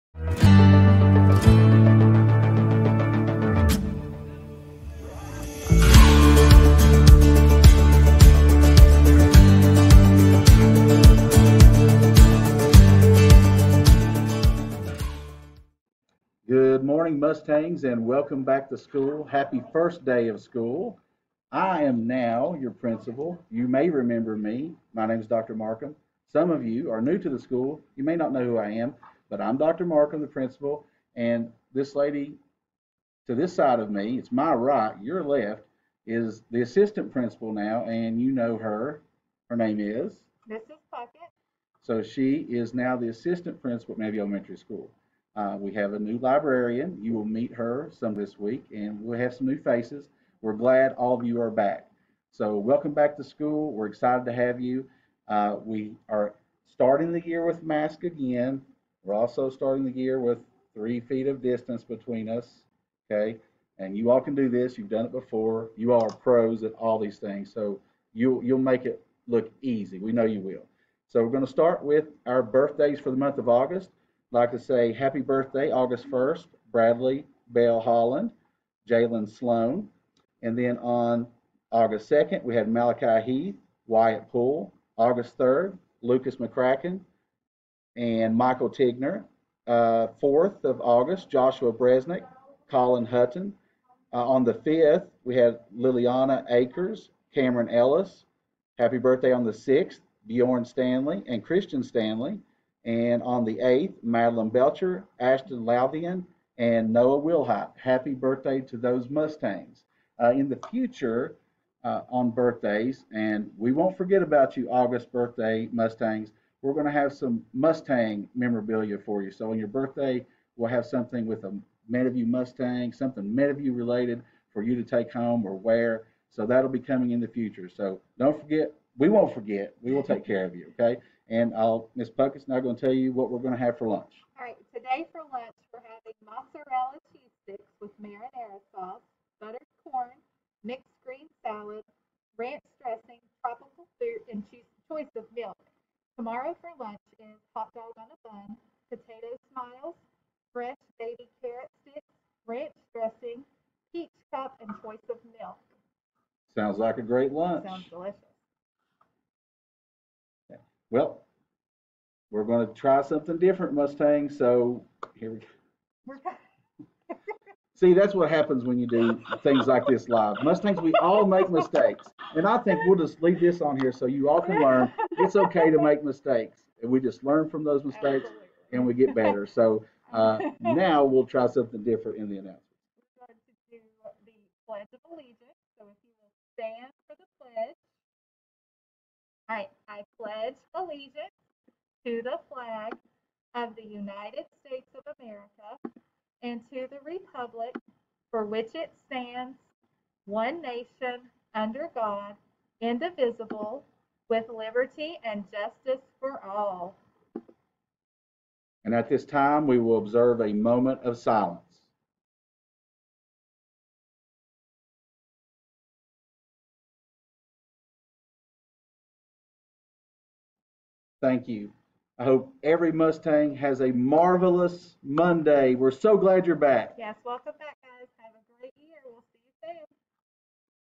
Morning Announcements--Monday, August 9, 2021
Morning Announcements for Monday, August 9, 2021 for Meadowview Elementary.

Welcome back to school, Mustangs!